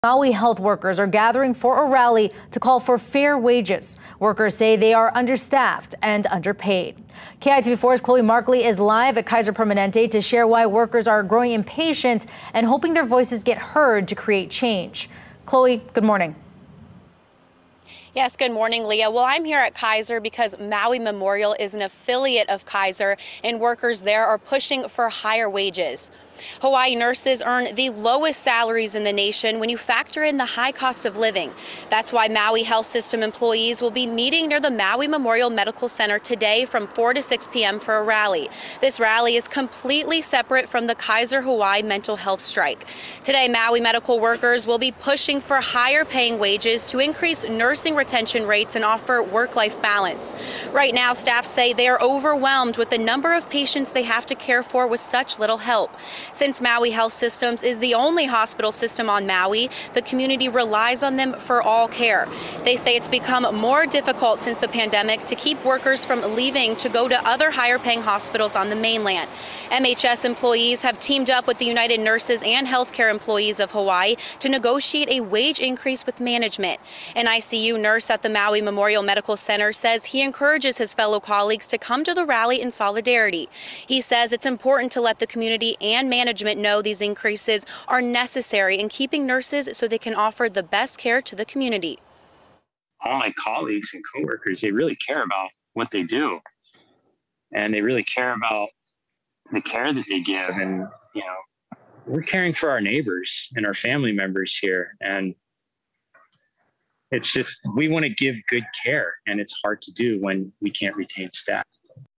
Maui healthcare workers hosting a rally to call for fair wages
Maui healthcare workers are gathering for a rally, Wednesday, to call for fair wages as they continue to be understaffed and underpaid.